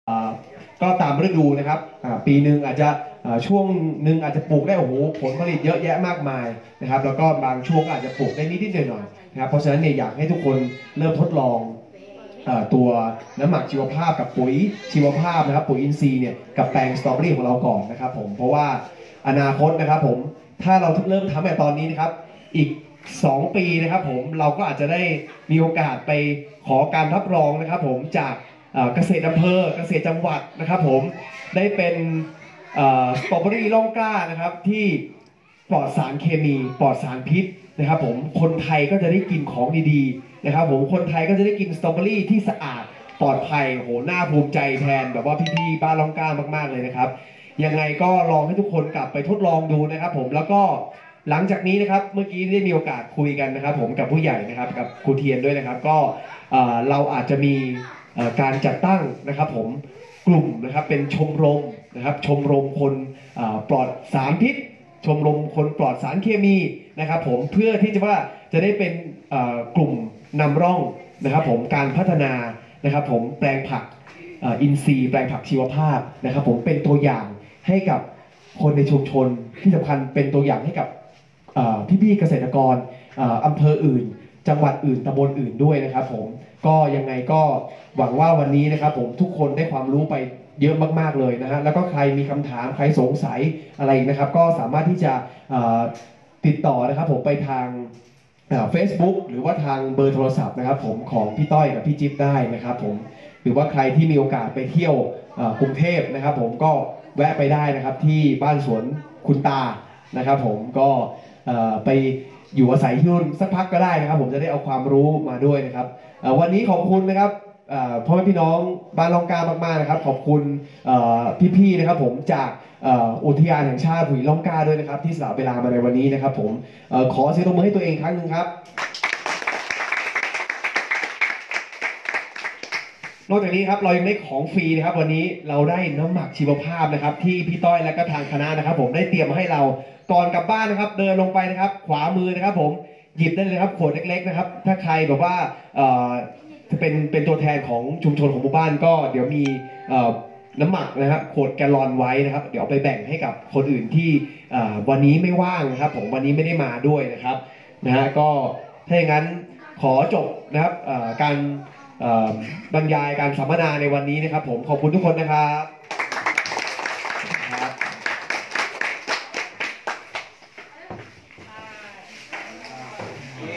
จุดประกายเกษตรแบบอินทรีย์ร่องกล้าสตรอว์เบอร์รีและผักปลอดสารพิษ By ณเดชน์ คูกิมิยะ
❤️ จุดประกายเกษตรแบบอินทรีย์
ร่องกล้าสตรอว์เบอร์รีและผักปลอดสารพิษ
By ณเดชน์ คูกิมิยะ @โรงเรียนบ้านห้วยน้ำไซ สาขาร่องกล้าวิทยา & บ้านร่องกล้า จังหวัดพิษณุโลก
22 /03/62